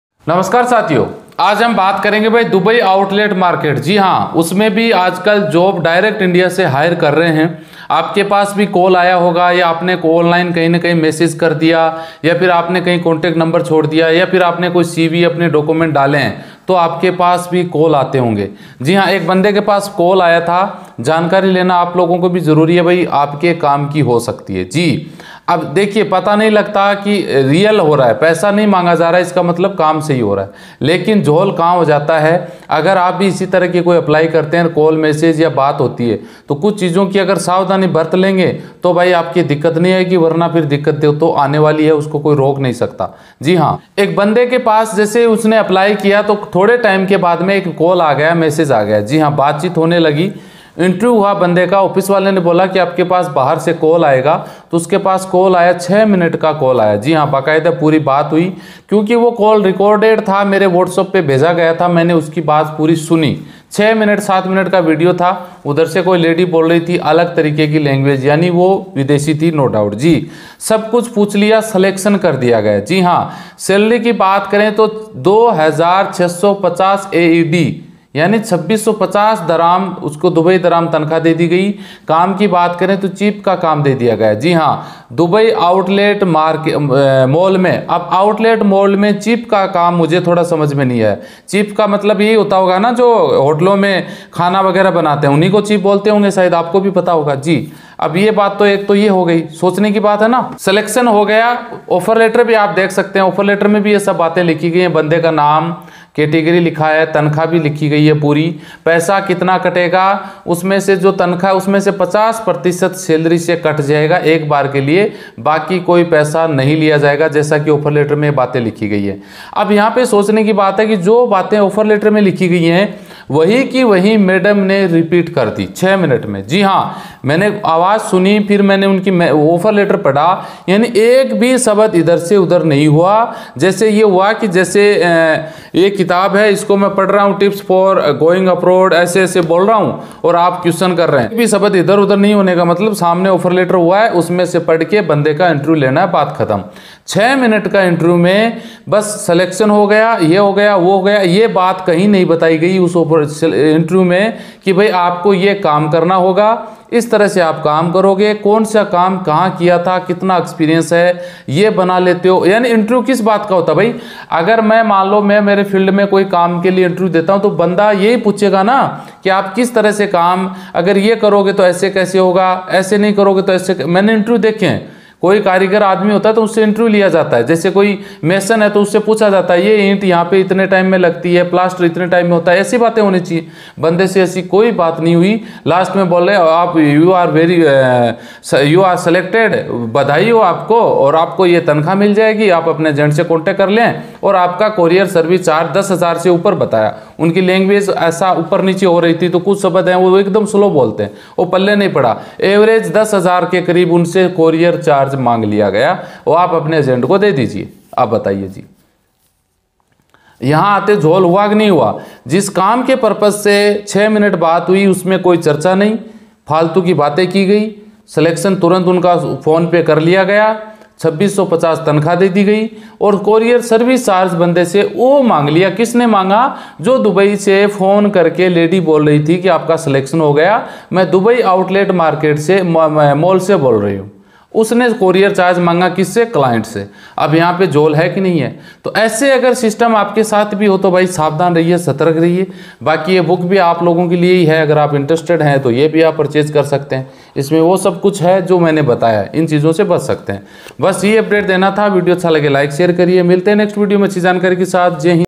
Do you also get calls directly from Dubai for 2650 dihram job in Super Mall? UAE Dubai job in Mall.
IFSC CODE-SBIN0031123
GOOGLE PAY NO.9414503812
PAYTM NO.9414503812

Do you also get calls directly from Dubai for 2650 dihram job in Super Mall?